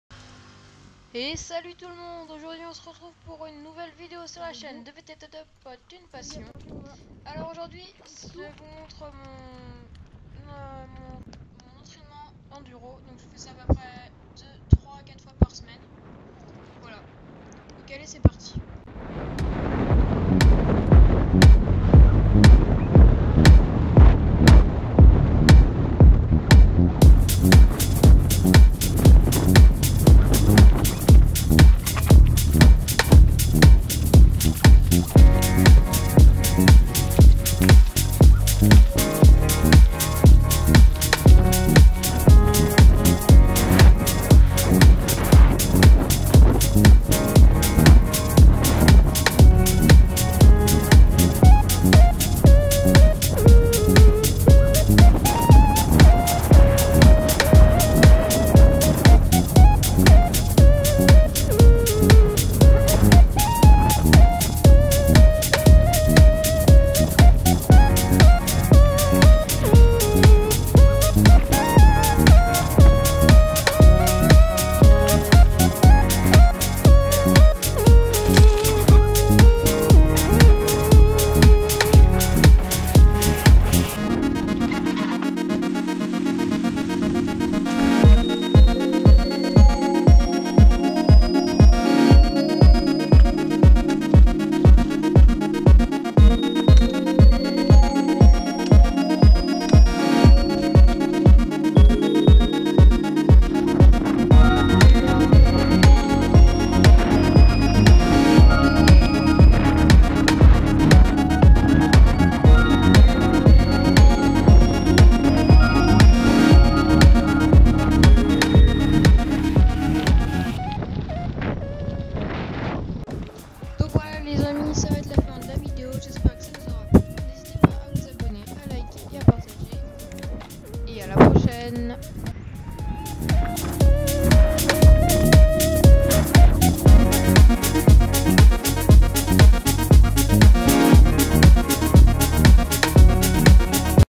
ENDURO TRAINING !!!!
Salut les amis c'est Etienne et on se retrouve pour une petite vidéo de mon entrainement de VTT enduro, .
Bonne vidéo
camera: